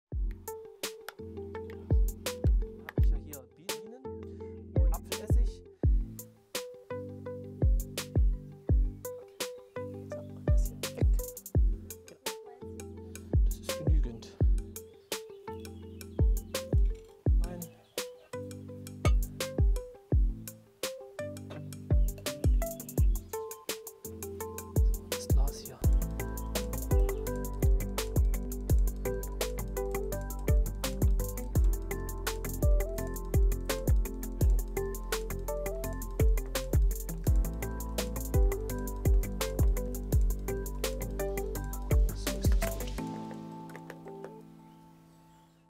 Bitter-Oxymel - Sauerhonig für die Verdauung
Die Zichorie, auch unter dem Namen Wegwarte bekannt, ist eine trockenheitsverträgliche, sehr vielseitige Pflanze. Strebergärtner Oliver baut sie bei sich im Garten an und bereitet sich daraus ein Bitteroxymel zu. Das ist ein Honig-Essig-Gemisch, dass die Verdauung unterstützen soll.

Zichorien-Samen sind übrigens tolle Winternahrung für Vögel.